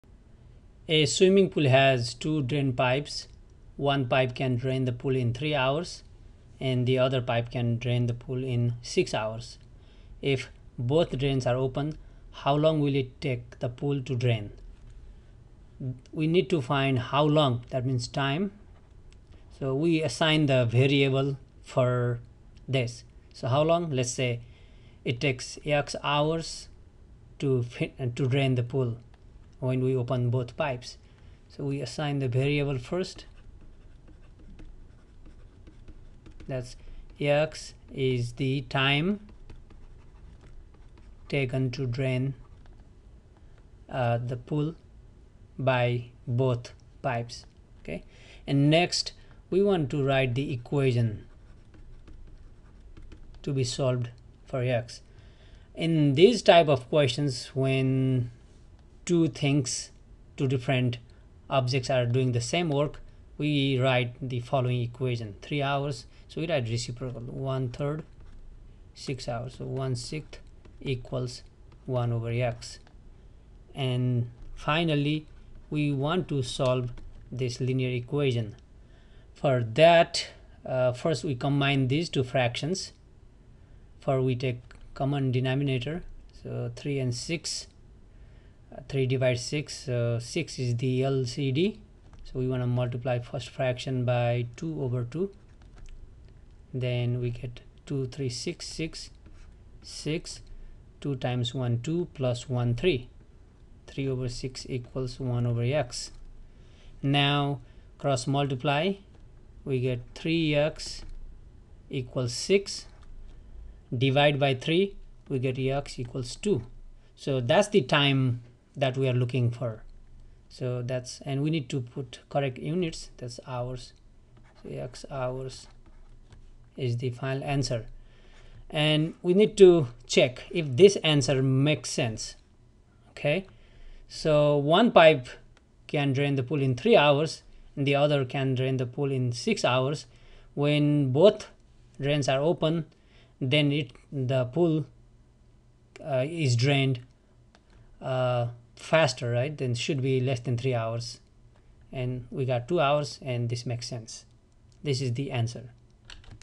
draining a pool using two pipes (college algebra)
A swimming pool has two drain pipes. One pipe can drain the pool in 3 hours and the other pipe can drain the pool in 6 hours. If both drains are open, how long will it take the pool to drain?